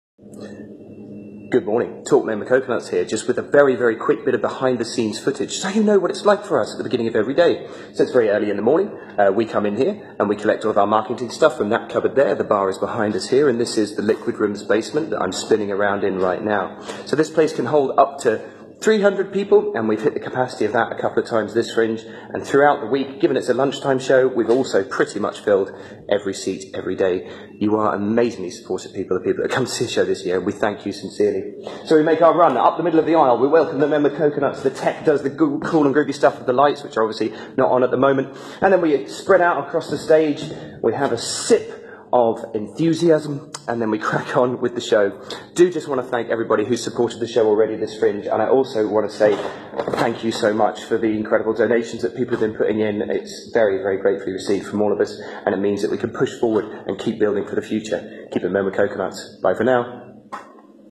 Talk Men With Coconuts -Ed Fringe 2018
Charlie giving you the lowdown on what goes on just before a show